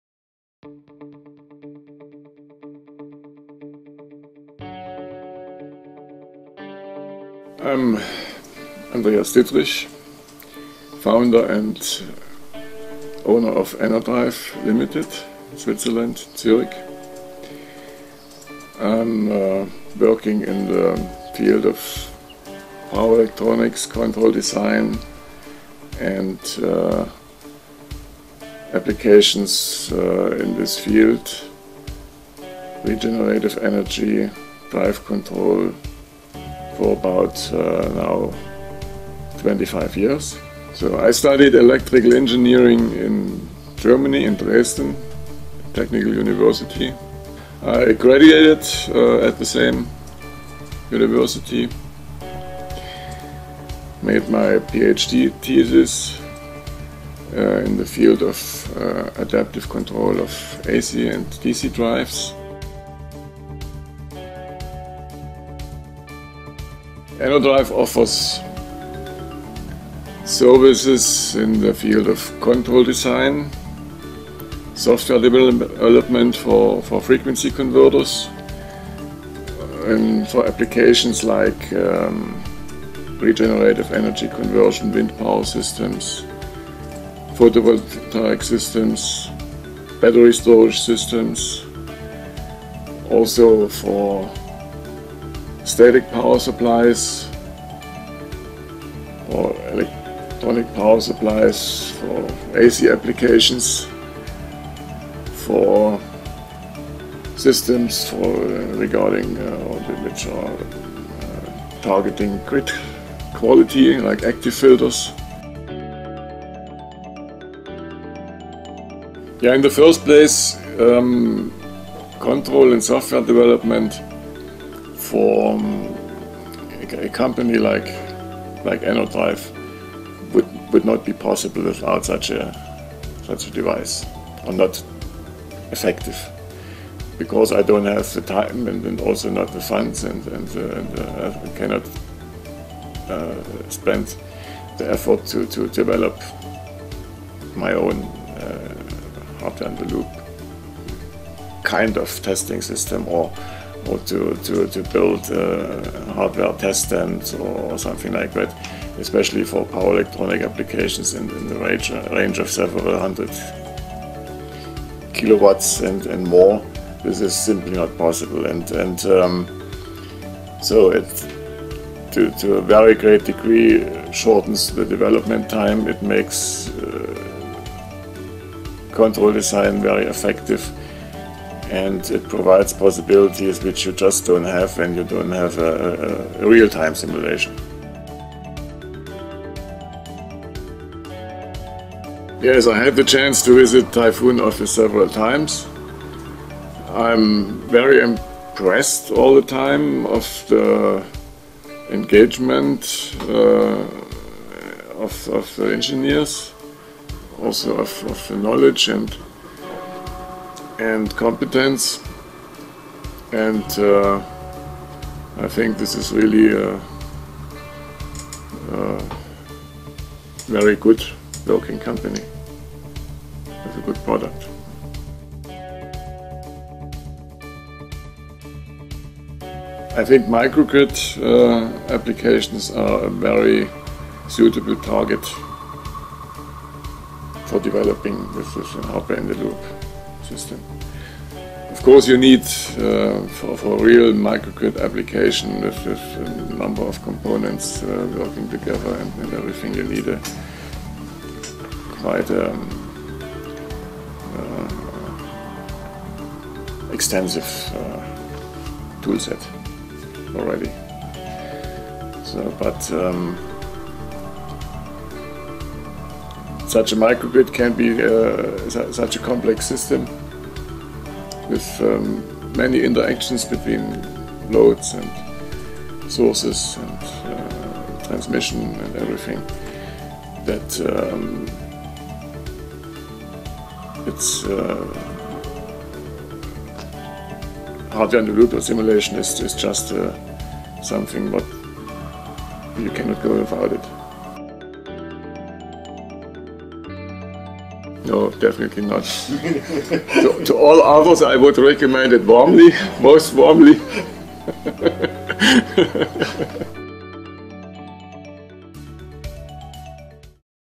Interview with Dr. Andreas Dittrich | Enerdrive GmbH Spotlight - teaser
Dr. Andreas Dittrich, founder and CEO of Enerdrive GmbH, original